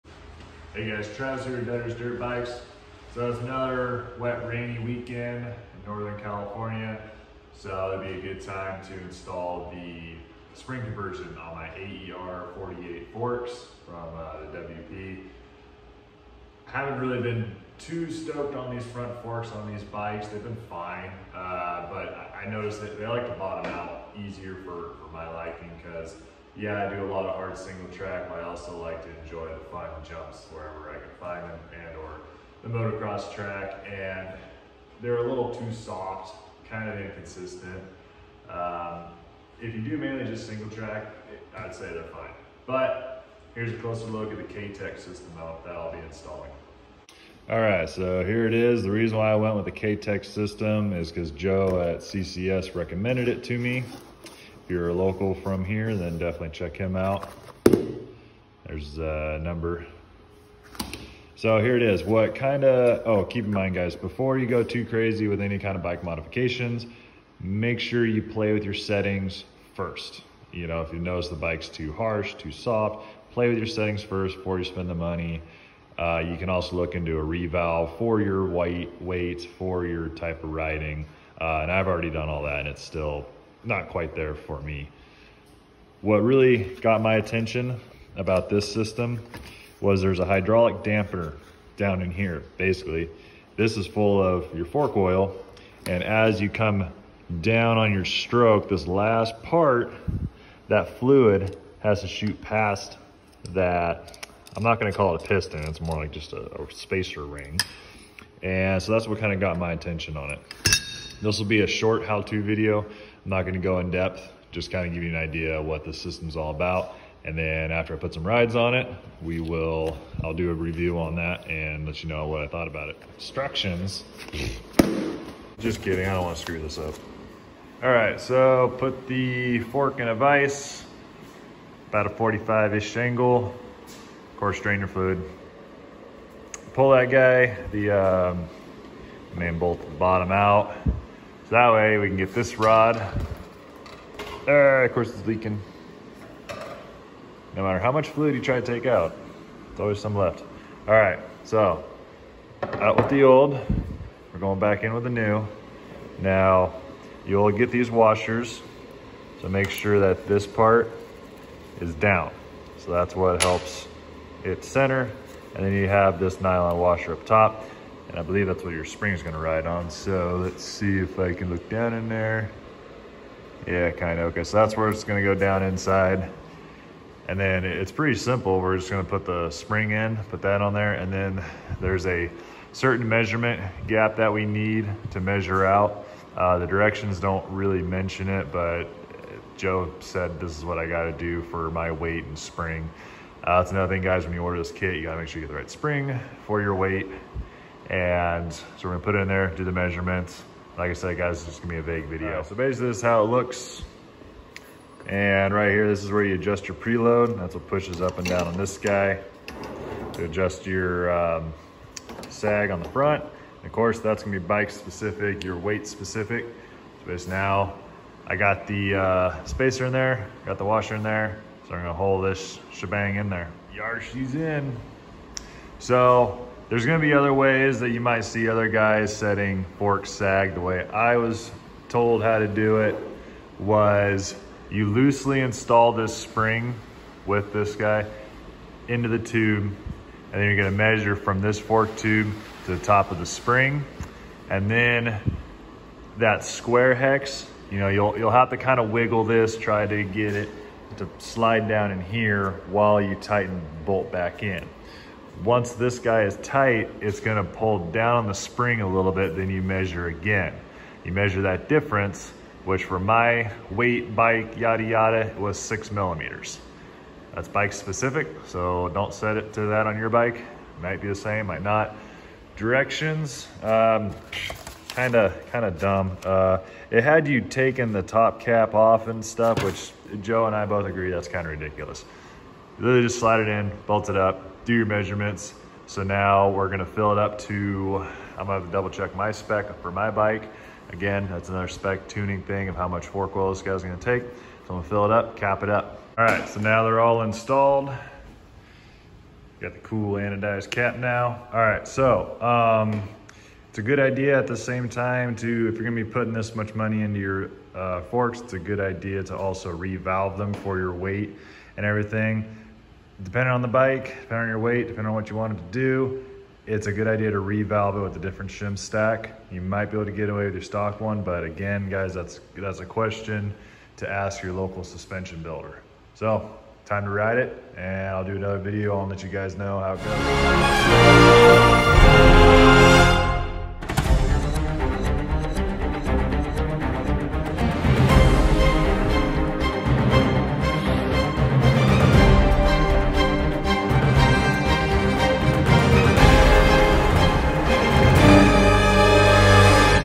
K-tech spring conversion unboxing and install
Review coming soon on converting my aer48 wp fork on my fx350 17 from air to spring from k-tech bought from ccs